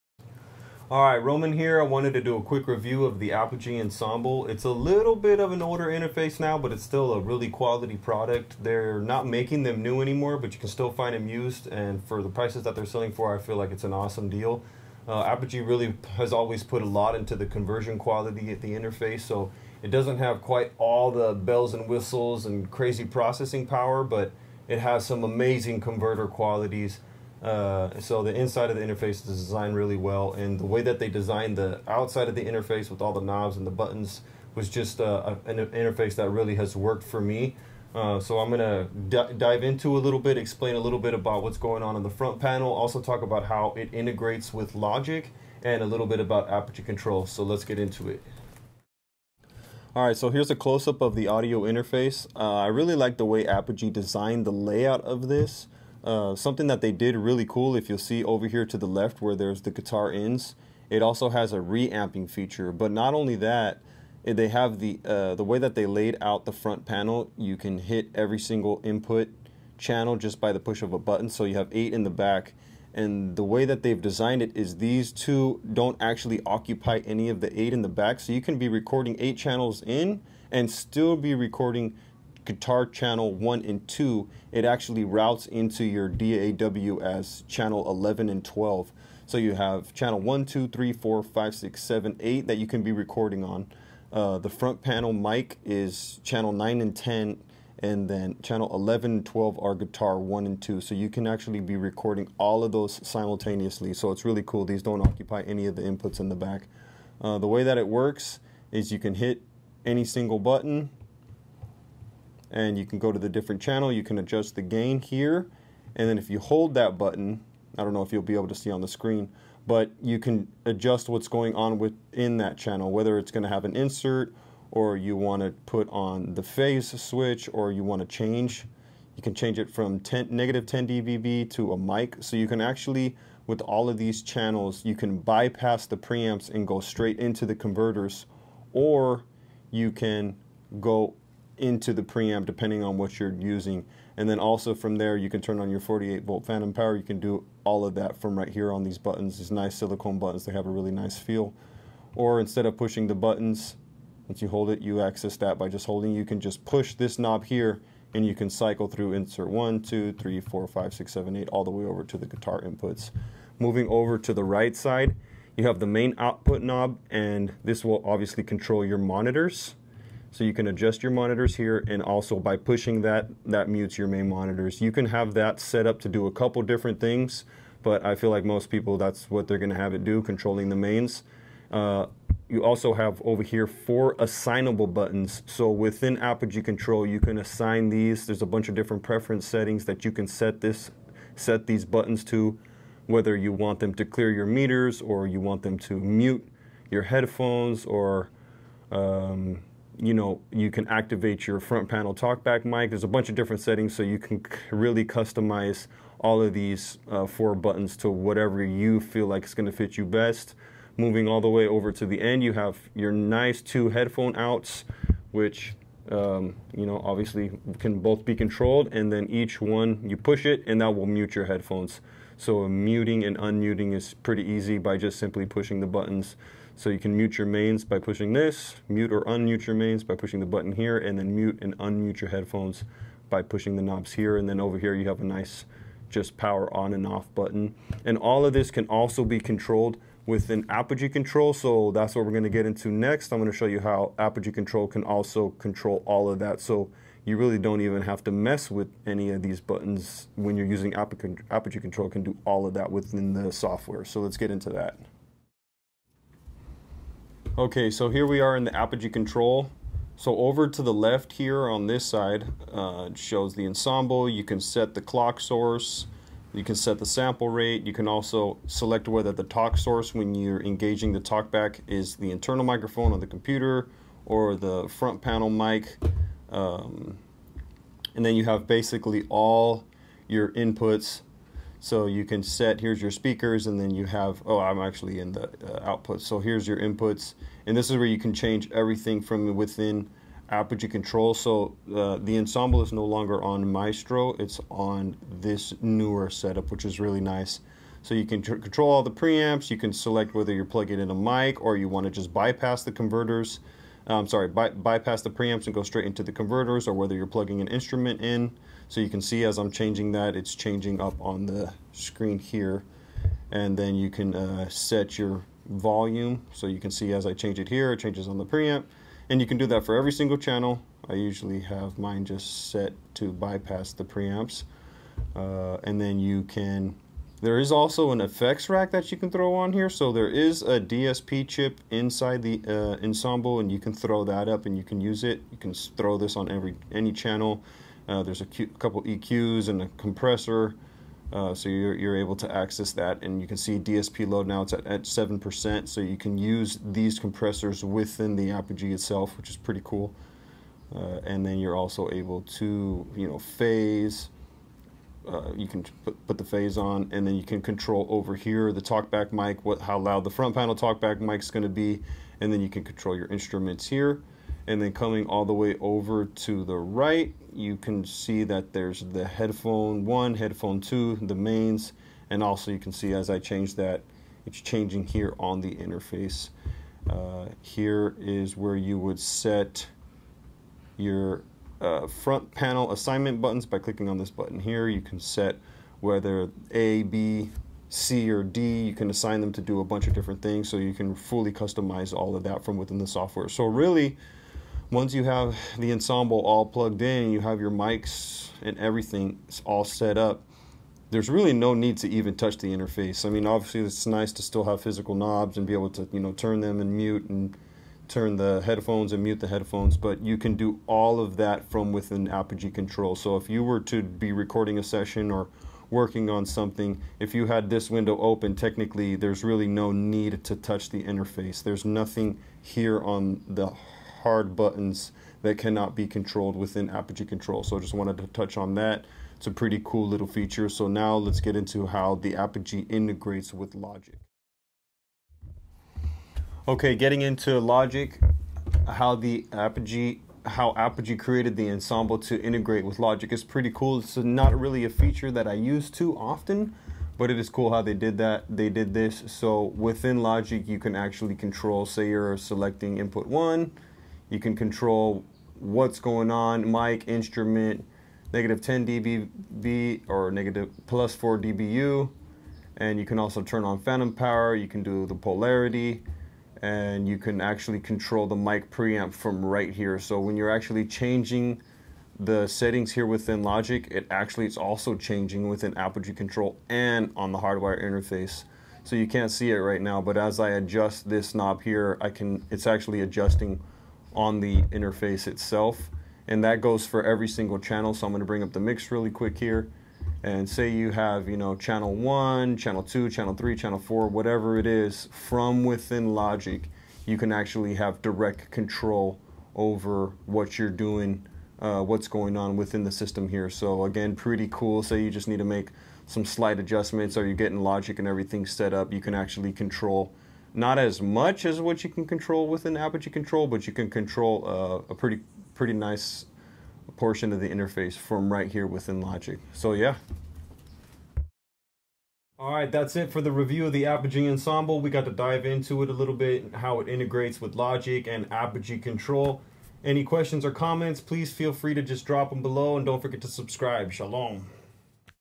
Apogee Ensemble thunderbolt (Review) Apogee control and integration with Logic
Apogee Ensemble thunderbolt (Review) Apogee control and integration with Logic. Hardware layout